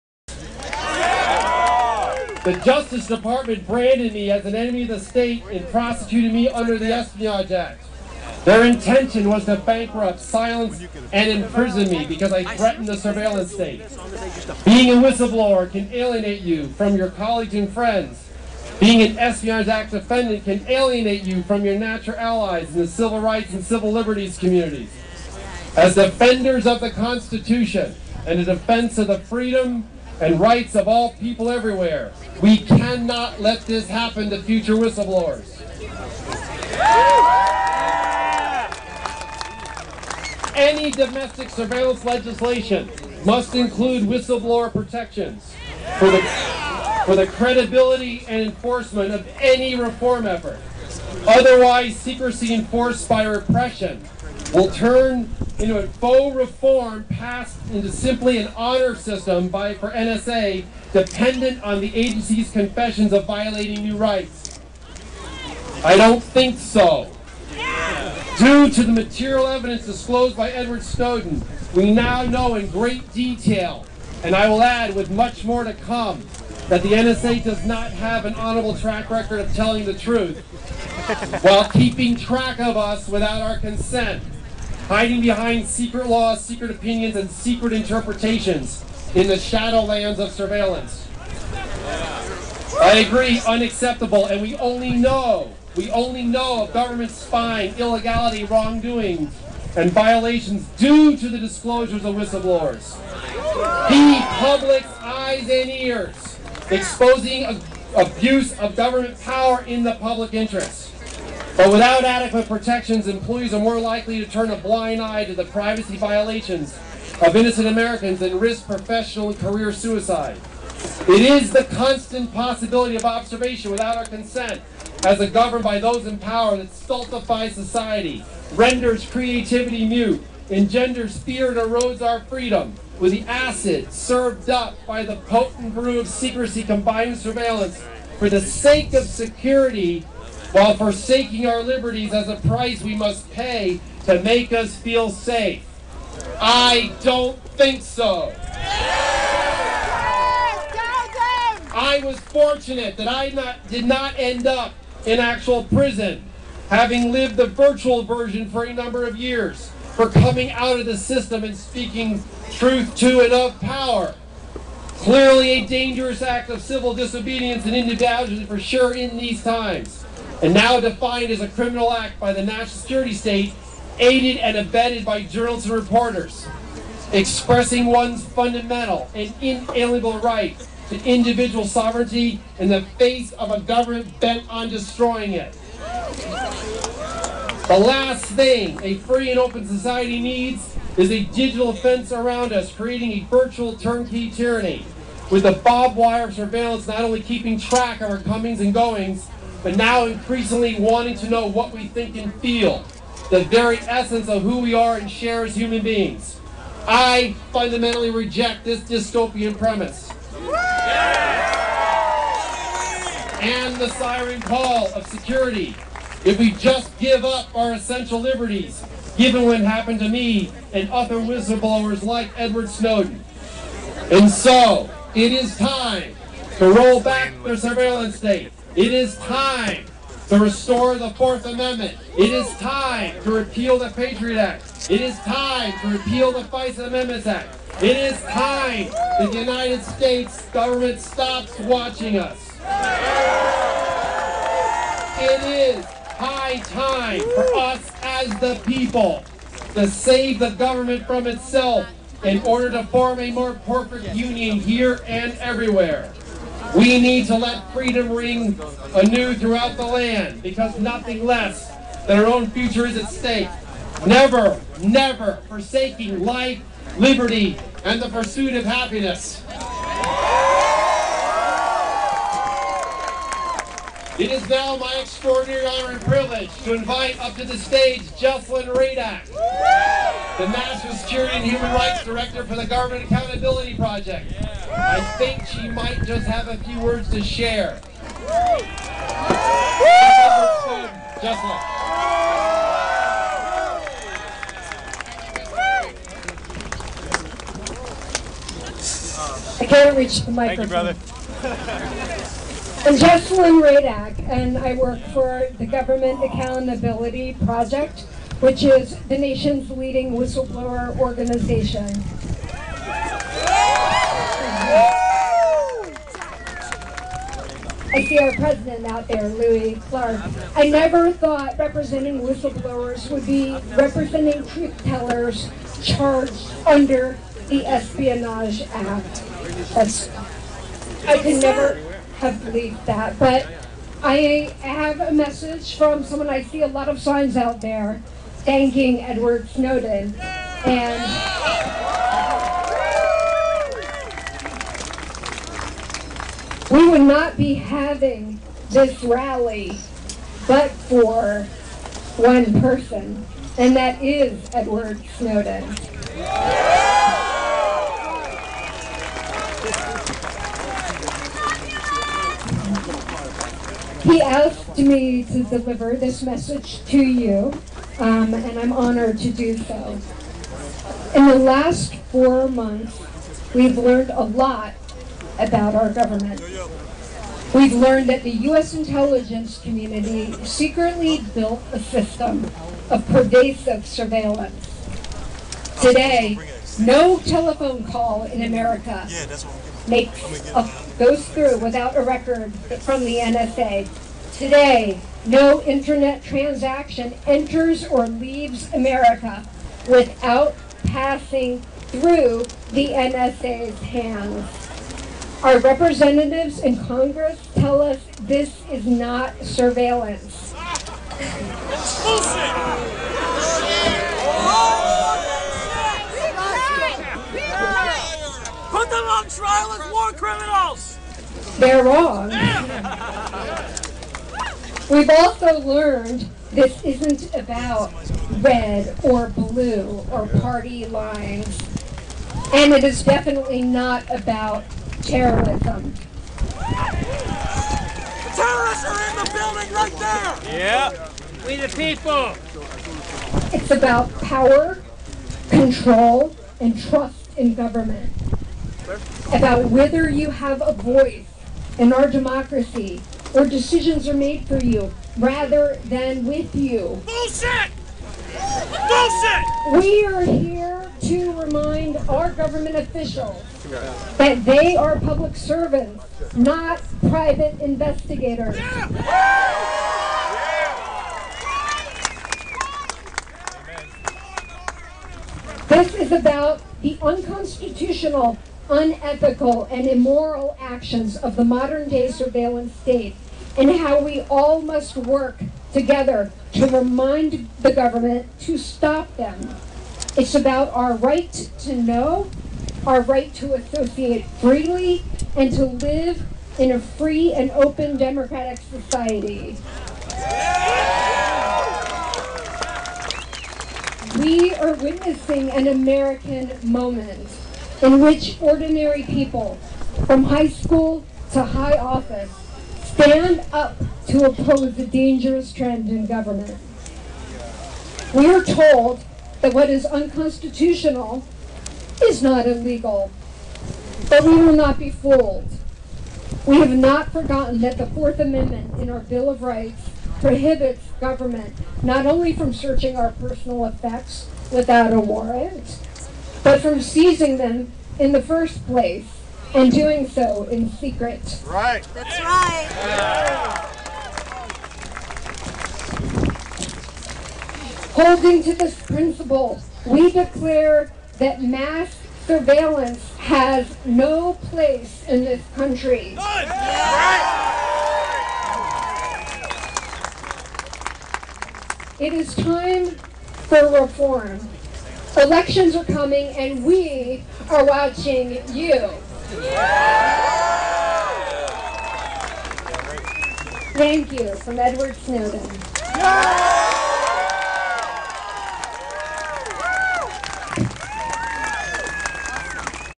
NSA Rally Thomas Drake and Jesselyn Radack Speak 10-26-2013
Thomas Drake, former NSA executive turned whistle blower and Jesselyn Radack, former ethics advisor to the US Dept of Justice, speak at the rally against NSA surveillance in Washington, DC on Oct 26, 2013.  Mr. Drake highlights the need for inclusion of whistle blower protection in reform of NSA surveillance.  Ms Radack read a statement from Edward Snowden, the whistle blower who exposed the classified NSA surveillance program known as PRISM.